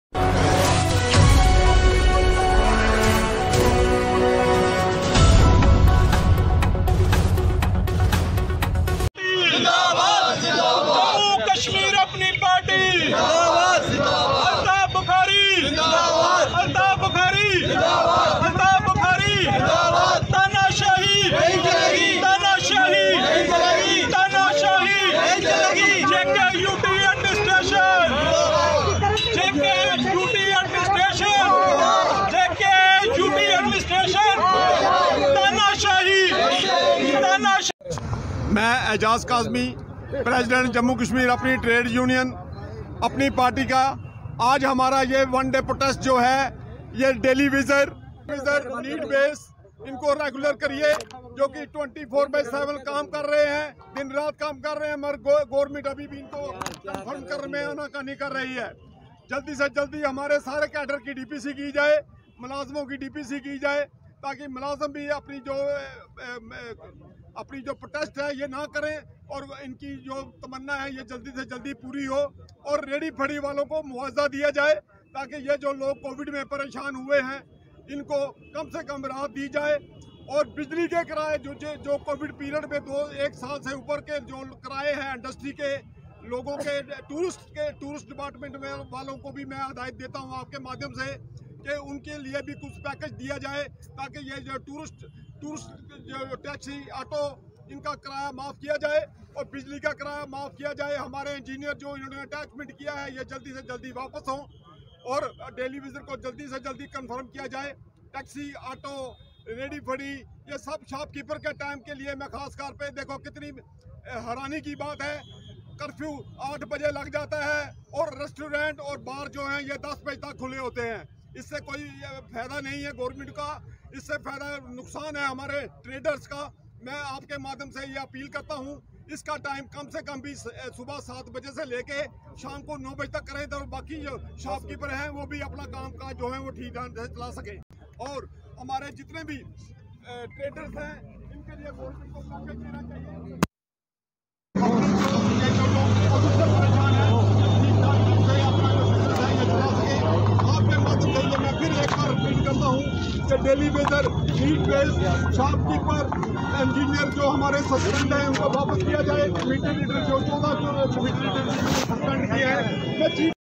Protest by Apni Trade union, tomorrow, against LG Admin. at Gandhiji statue satwari Chowk JMU/FNN
Protest by Apni Trade union, tomorrow, against LG Administration at Gandhiji  statue satwari Chowk, Jammu